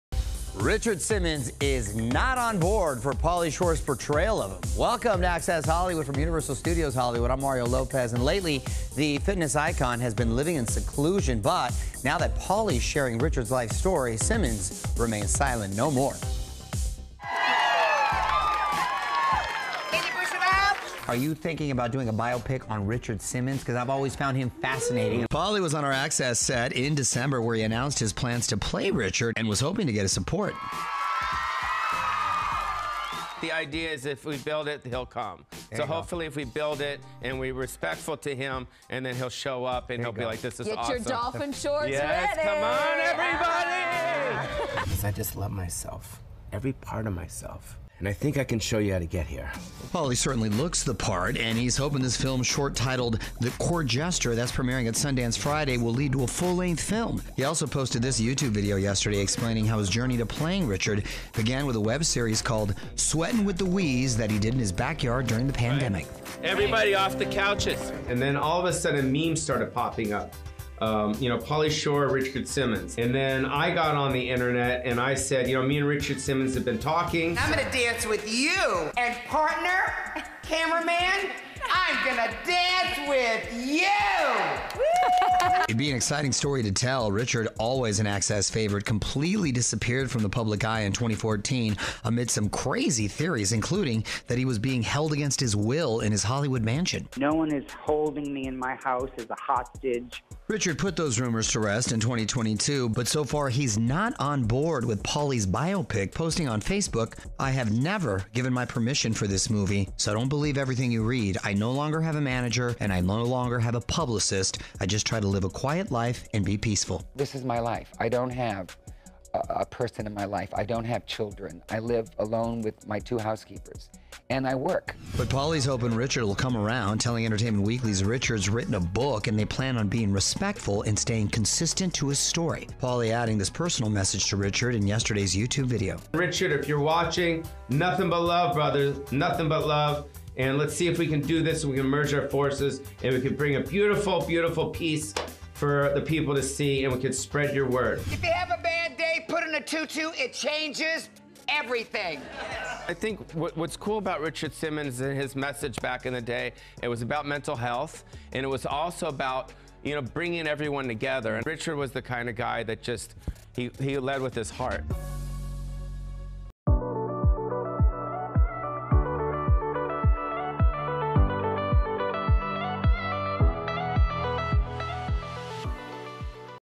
Richard Simmons Makes Rare Statement On Pauly Shore Playing Him In Upcoming Biopic
Richard Simmons is not on board for an upcoming biopic about himself starring Pauly Shore. The fitness icon has been out of the public eye for years, but he recently took to Facebook to set the record straight on his feeling about the project. "I have never given my permission for this movie. So don't believe everything you read," he wrote in part. "I no longer have a manager, and I no longer have a publicist. I just try to live a quiet life and be peaceful." Pauly, however, is hoping Richard will come around, telling Entertainment Weekly that he and the filmmakers plan on being respectful and "consistent" to Richard's book.